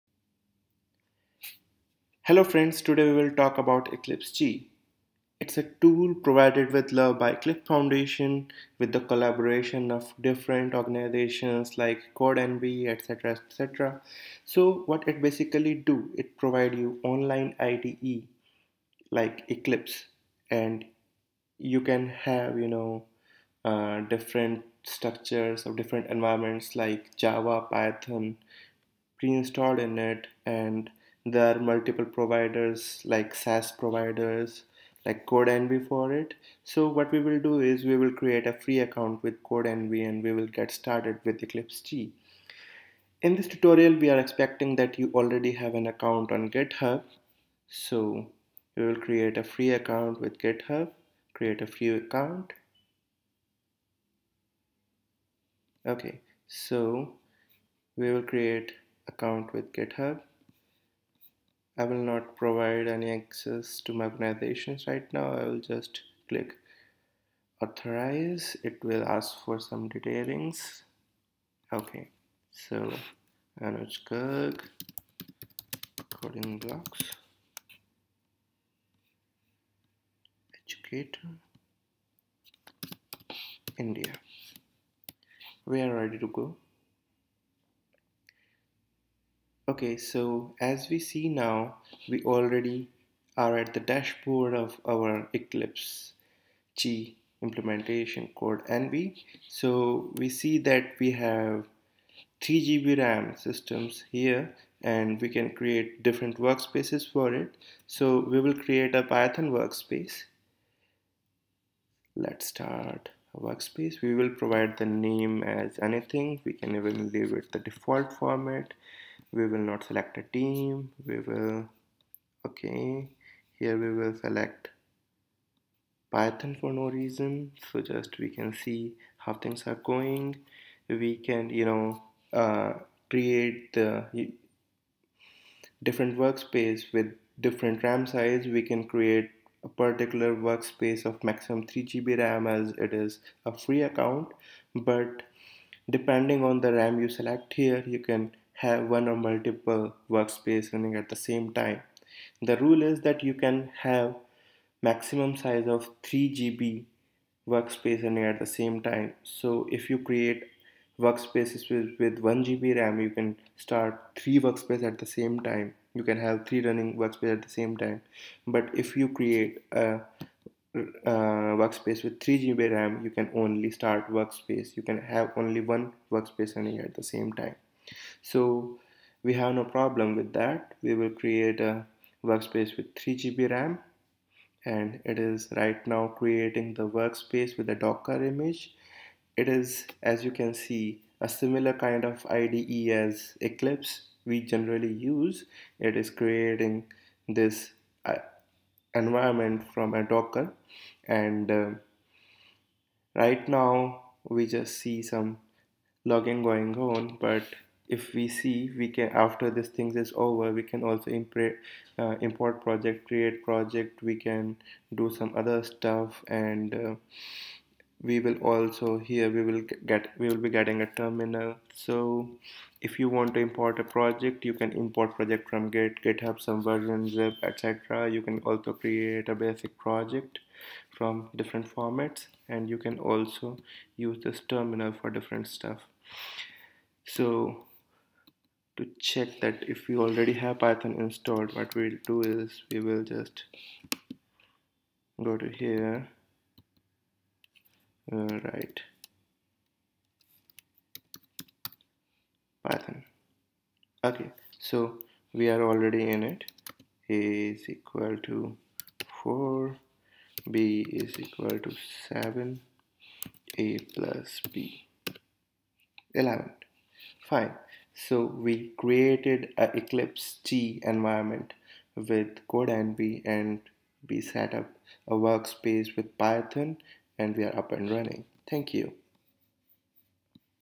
Getting started with Eclipse Che and Codenvy
Setting up ide and platform is big hassel for someone just getting to start with coding or trying code on the go.  Eclipse che is here for your rescue.